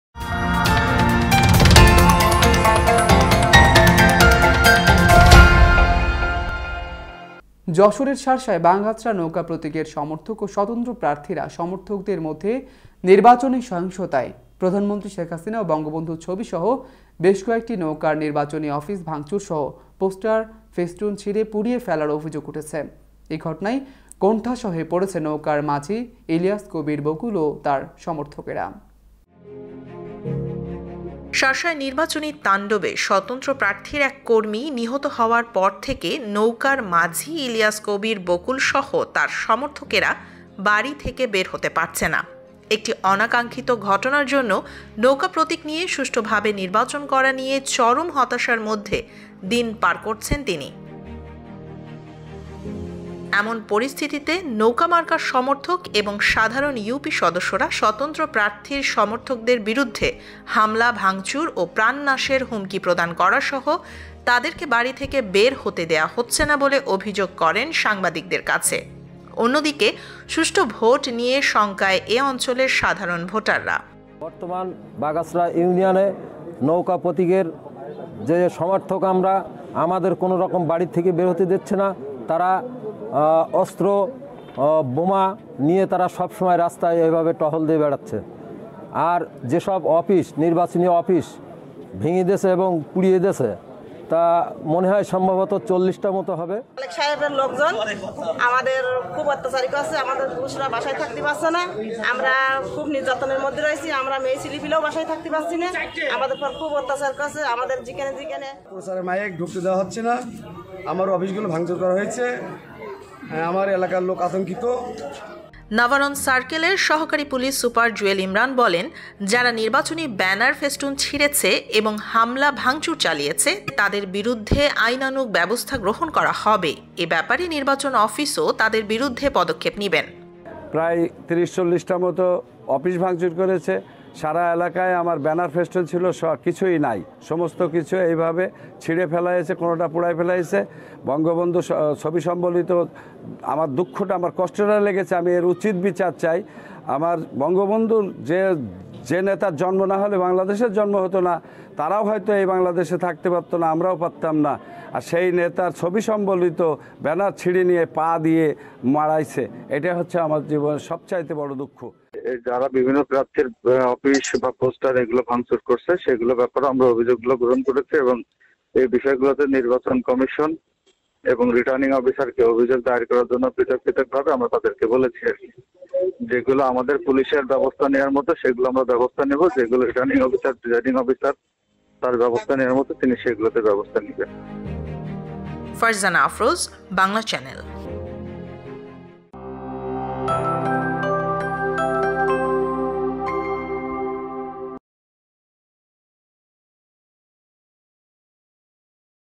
শার্শার বাগআঁচড়ায় নির্বাচনী সহিংসতায় প্রধানমন্ত্রী ও বঙ্গবন্ধুর ছবিসহ বেশ কয়েকটি নৌকা ভাংচুর
🌟 বিস্তারিত: যশোরের শার্শার বাগআঁচড়ায় নৌকা প্রতীকের সমর্থক ও সতন্ত্র প্রার্থীর সমর্থকদের মধ্যে নির্বাচনী সহিংসতায় প্রধানমন্ত্রী শেখ হাসিনা ও বঙ্গবন্ধুর ছবিসহ বেশ কয়েকটি নৌকার নির্বাচনী অফিস ভাংচুরসহ পোস্টার ফেস্টুন ছিড়ে পুড়িয়ে ফেলার অভিযোগ উঠেছে।

🌟  যোগাযোগের ঠিকানা:
Bangla Channel